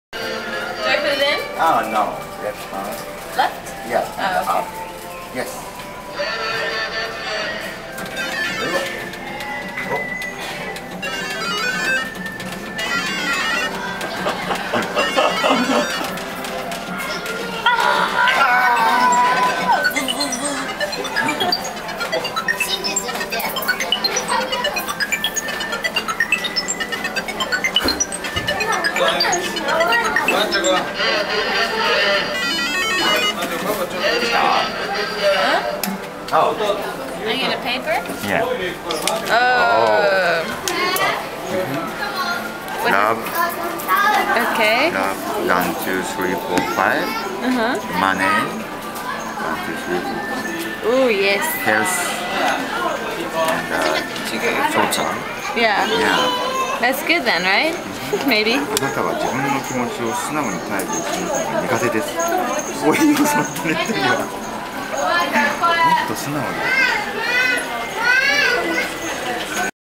kobe fortune
"Bocca Della Verità" ("The Mouth of Truth") fortune teller machine in kobe aquarium, kobe, japan, provides promising results